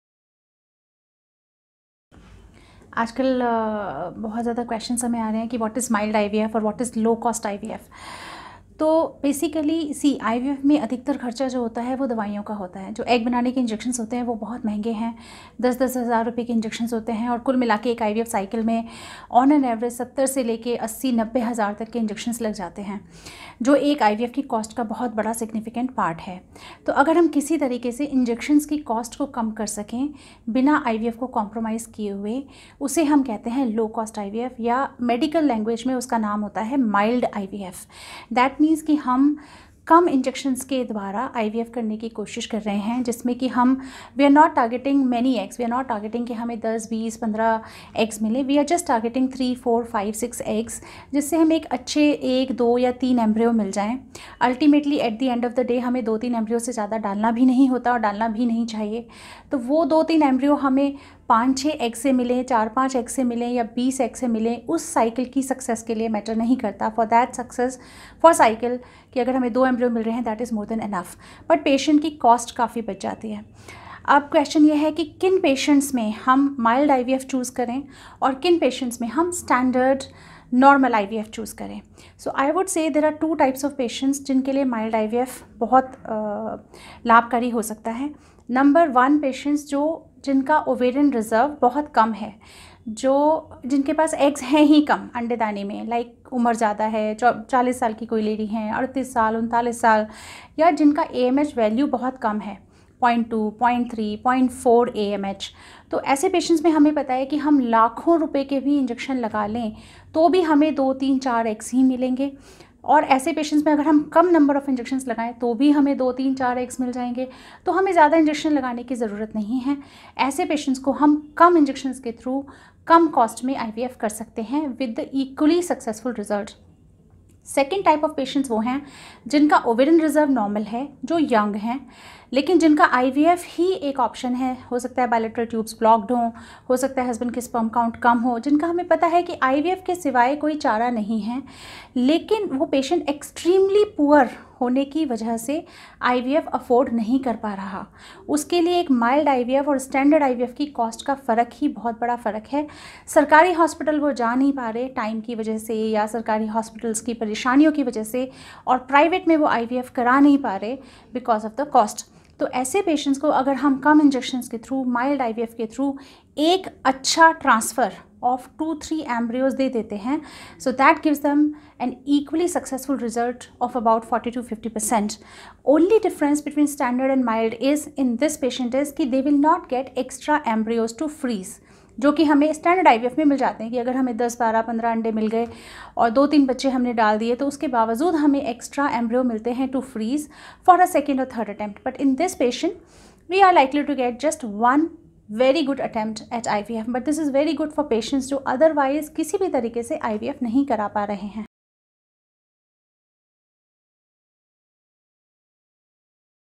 What is Low-Cost IVF Treatment in Delhi | Mild IVF Treatment in Delhi? - Hindi
If you want to know what is low-cost IVF treatment / Mild IVF Treatment in Delhi, you would actually get this at state of the art clinic with the best doctors and technicians handling your case.

Dr. Rashmi Sharma offers low-cost IVF Treatment, Infertility Treatment in Delhi.